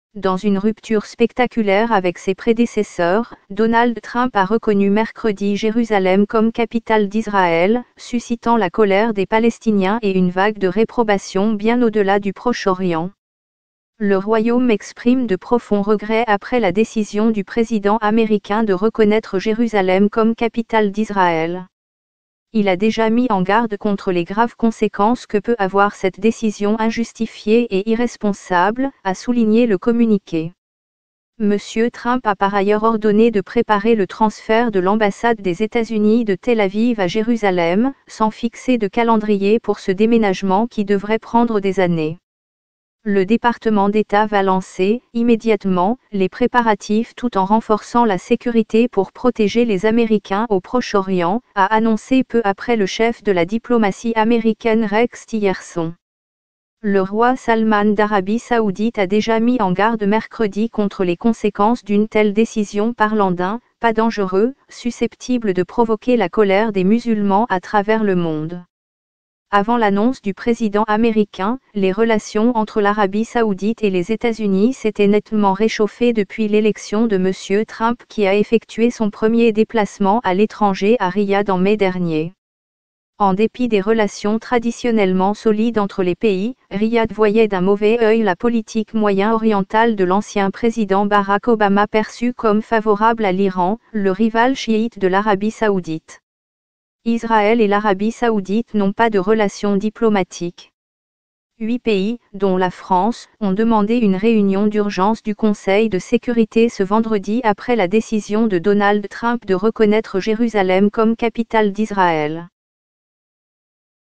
Jérusalem: Ryad condamne la décision de Trump
Dans une rupture spectaculaire avec ses predecesseurs, Donald Trump a reconnu mercredi Jerusalem comme capitale d'Israel, suscitant la colere des Palestiniens et une vague de reprobation bien au-dela du Proche-Orient.